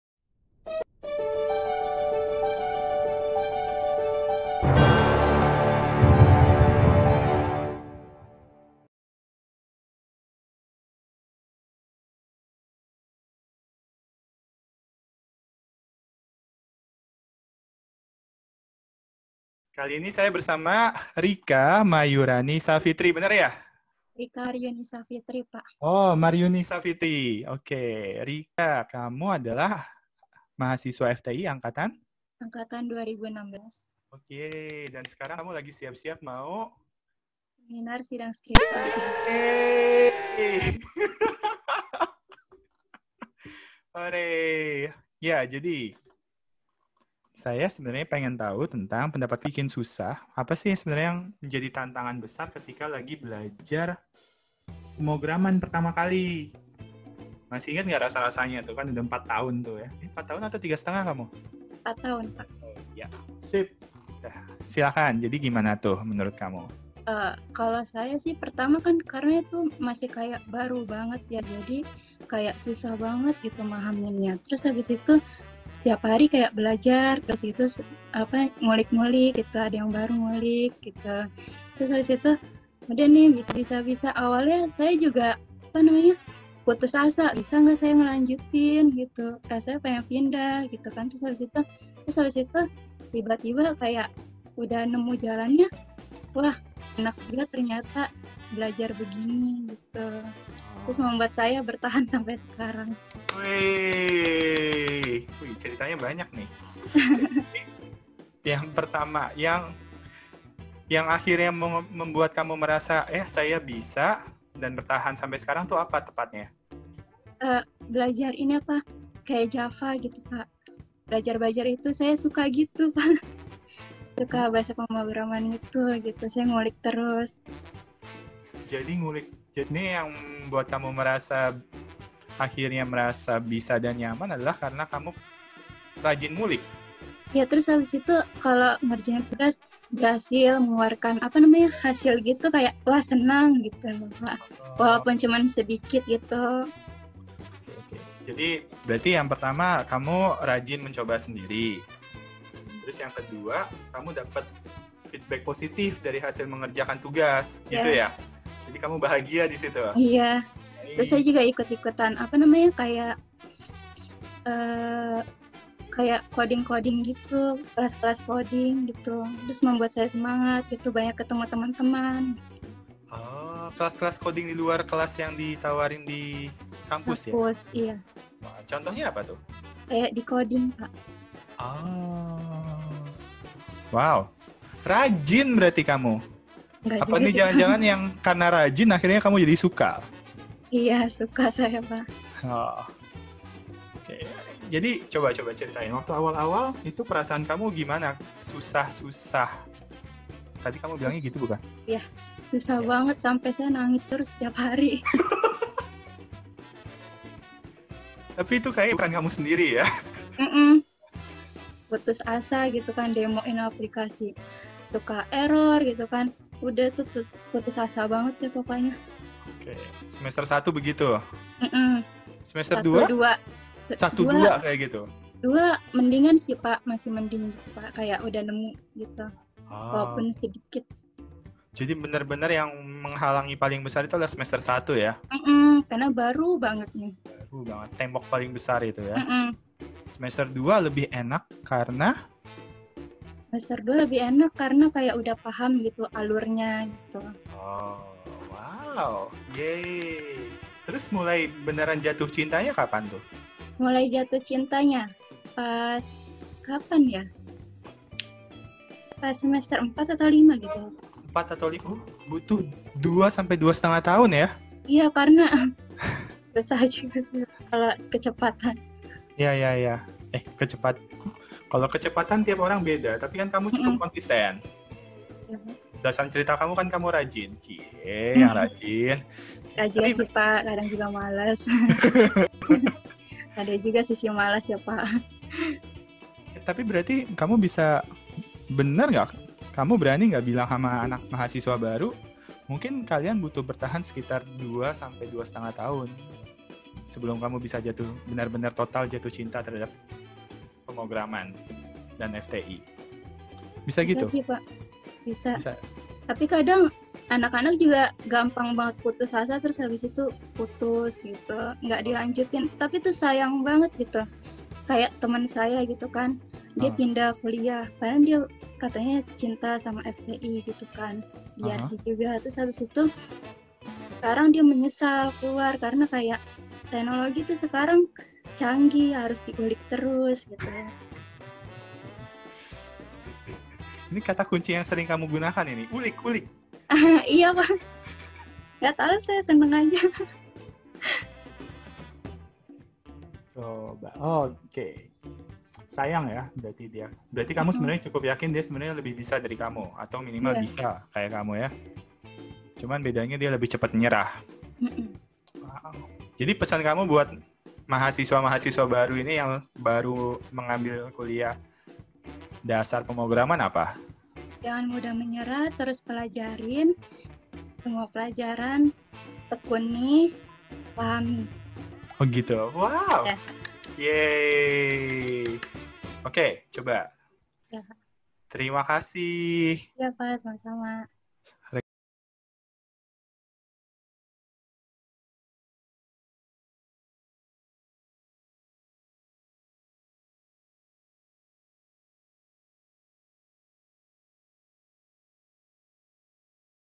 Ngobrol Yuk - Seri 1
Ngobrol Yuk!
Seri 1 - Bicara tentang Programming

Kali ini kami mengajak Rika Maryuni Safitri, mahasiswi Fakultas Teknologi Informasi sejak tahun 2016, untuk mengenang pengalamannya pertama kali belajar Bahasa Pemrograman (koding). Ternyata, Rika baru mengenal koding pada saat masuk kuliah, dan seperti kebanyakan orang, dia pun kesulitan. Rika pernah merasa sangat buruk (penuh tangisan) dan putus asa dalam belajar, tapi sekarang dia sudah jatuh cinta dengan koding. Pada 06:04, Rika menyarankan agar teman-teman yang sedang belajar programming di bangku kuliah untuk tidak mudah menyerah, terus pelajari, tekuni, dan pahami semua materi yang diberikan.

Yuk kita dengarkan lebih lengkap!